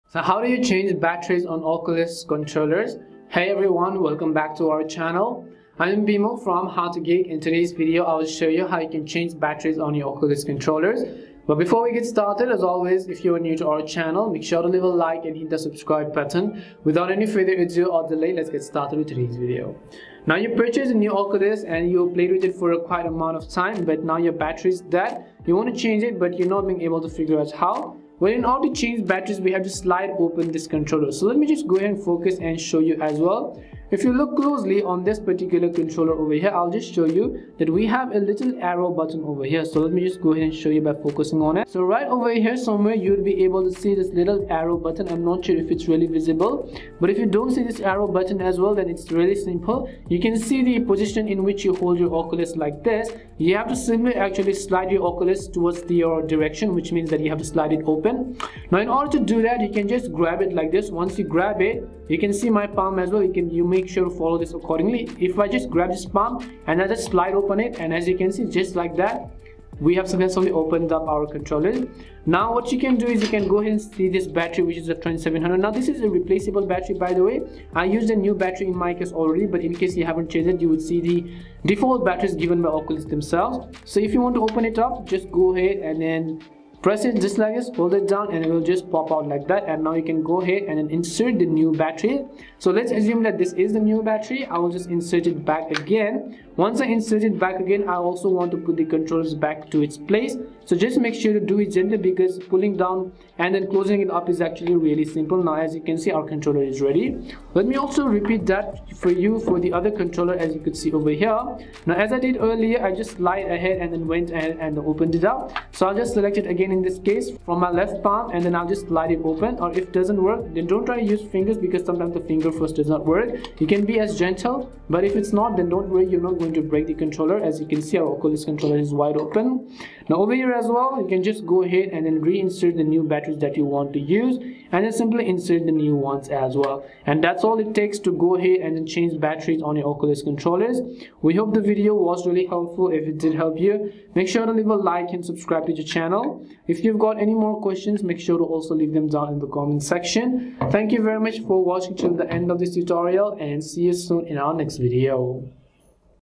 How Do You Change Batteries in Oculus Quest 2 Controllers
In this tutorial video, I will quickly guide you on how you can change batteries in Oculus controllers. So make sure to watch this video till the end. We try to be responsive to as many questions as possible.

The arrow icon should be pointing up as you hold the controller. Slide the cover over the battery recess up by applying a little pressure. Remove the cover to access the battery, then swap out the old one. Follow the same procedures for the second controller after sliding the cover into position.

~Timestamps:
0:00 Introduction
0:20 General Info
0:46 How Do You Change Batteries in Oculus Quest 2 Controllers
2:17 Conclusion